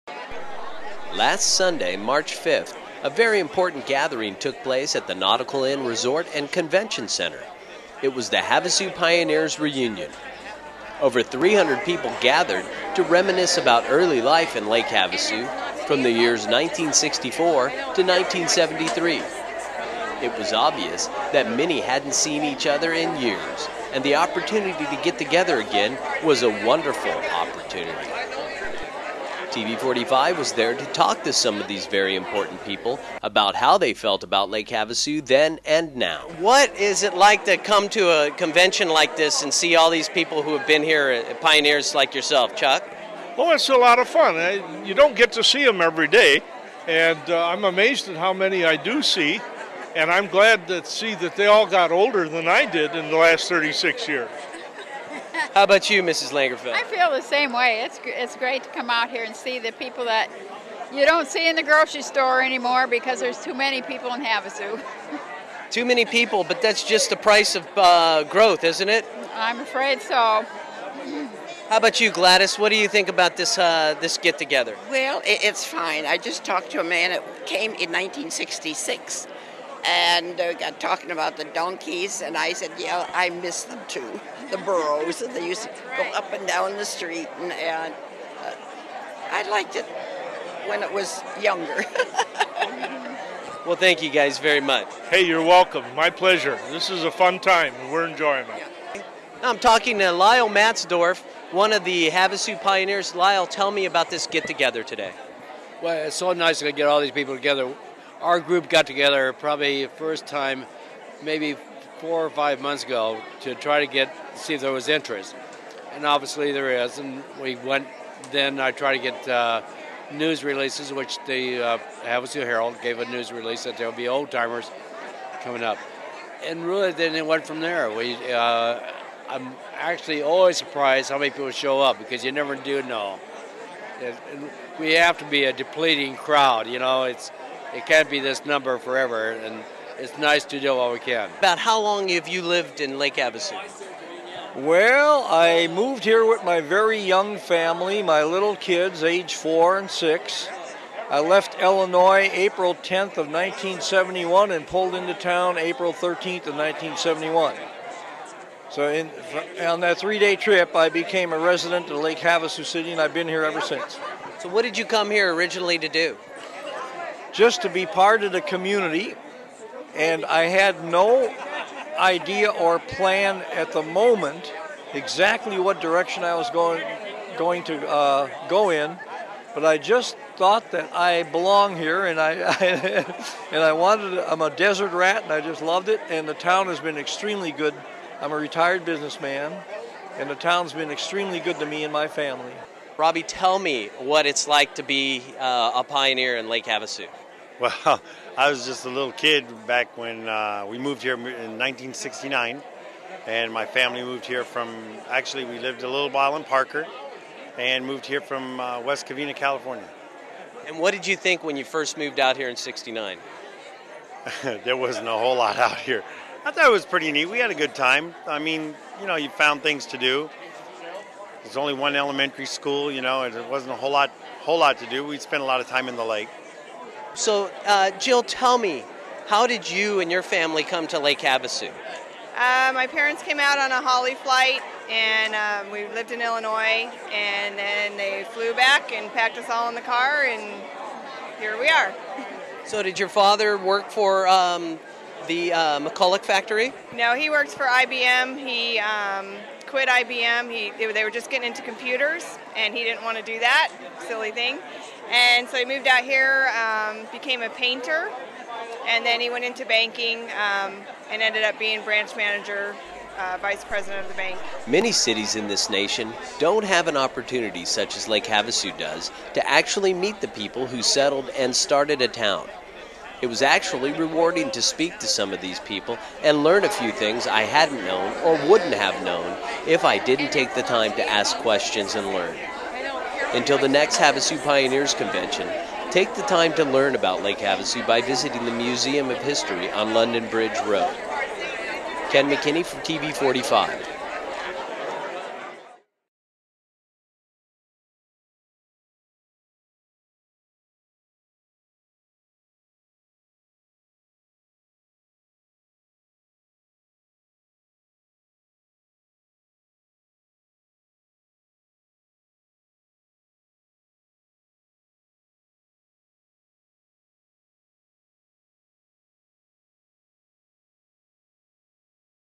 Havasu Pioneers Gather for Reunion
You know I really like Lake Havasu. I lived there for just a short time but had loved it since childhood as a great little vacation destination like no other. A Sothern California native, I lived out of the country for over 10 years and when I returned I wanted to return there. Well, I did. I was able to meet so many people during my brief stay there and some of the most interesting people I was able to meet were those that settled the town, the Havasu Pioneers. I was fortunate enough to be working at the TV station and thought they were important enough to cover as a news piece. You know I was the only reporter there! Well, I had a nice time getting to know those nice people. Here is my report.